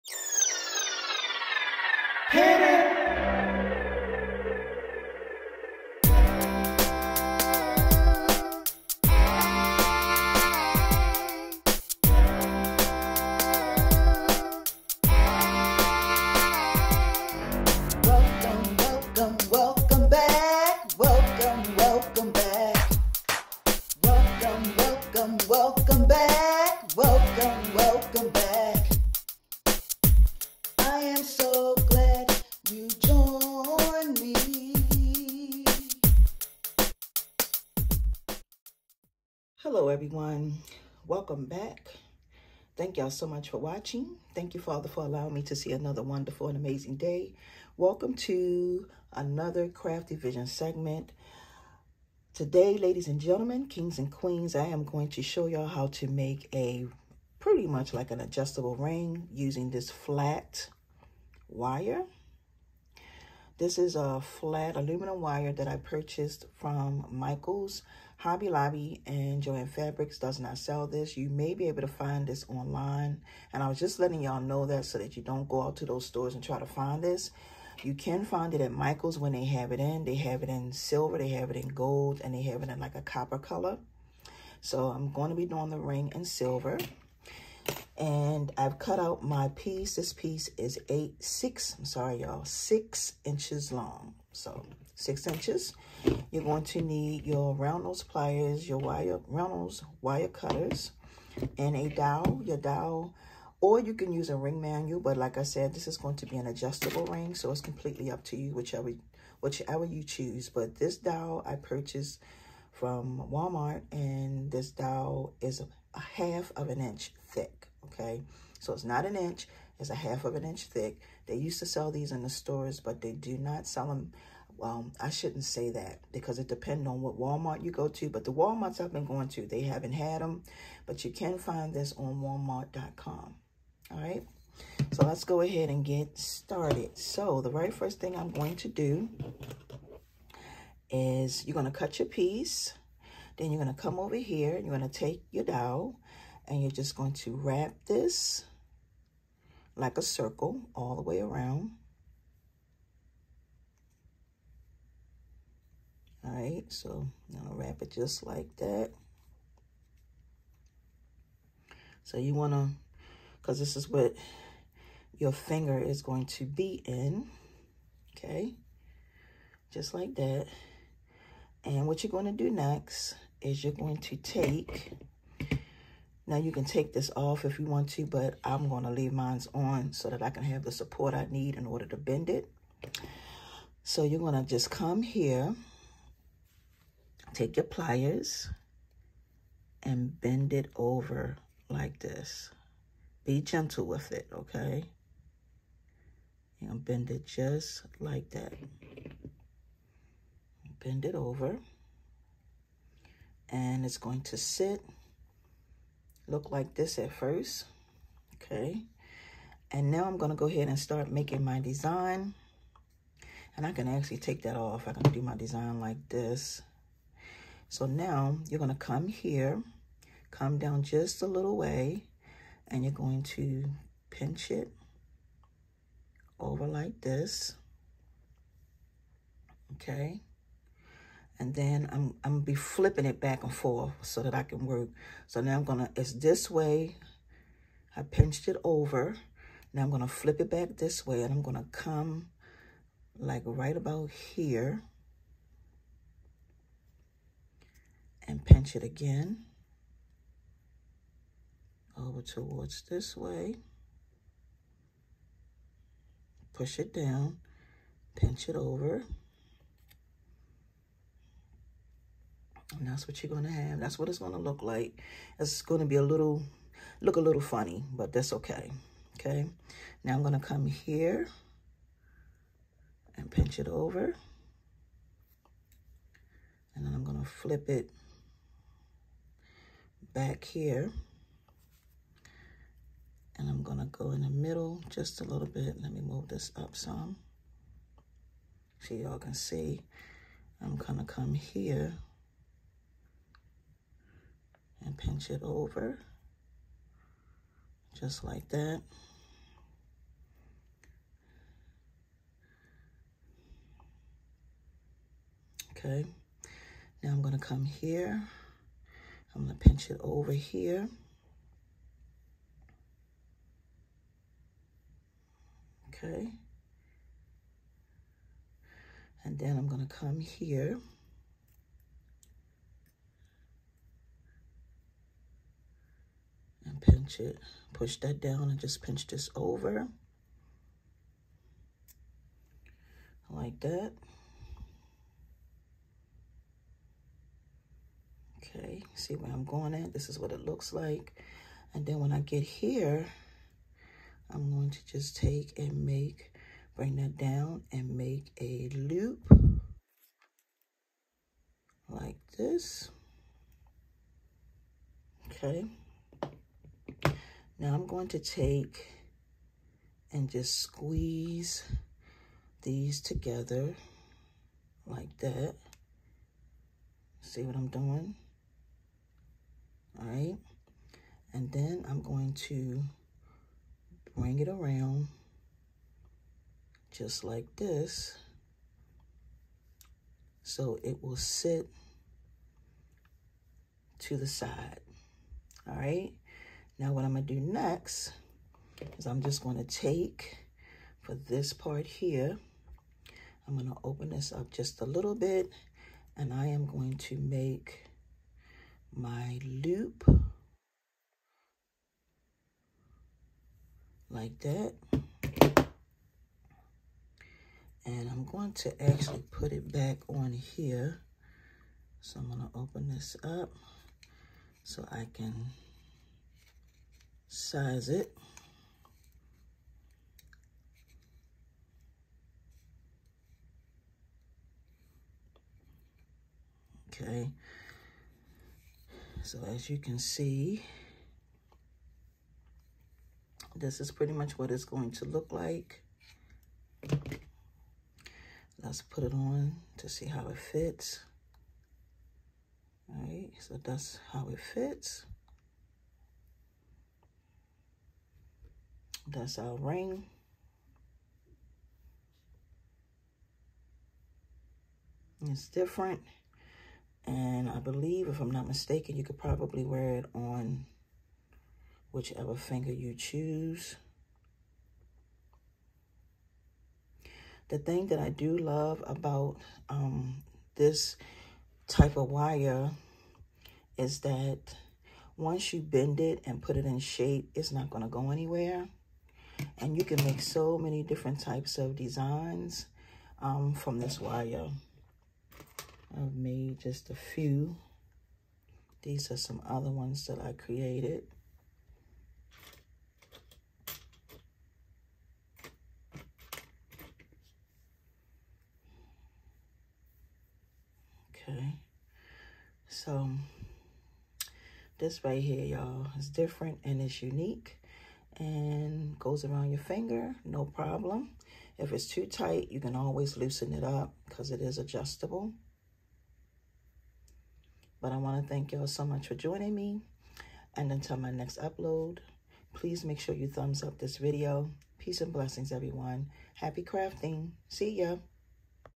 How to make adjustable ring, using flat aluminum wire
Hello everyone today I am going to show you all how to do a jewelry tutorial using flat aluminum wire. We are going to be creating a beautiful adjustable ring. You can find this wire at michaels . Have an amazing day everyone happy crafting see ya.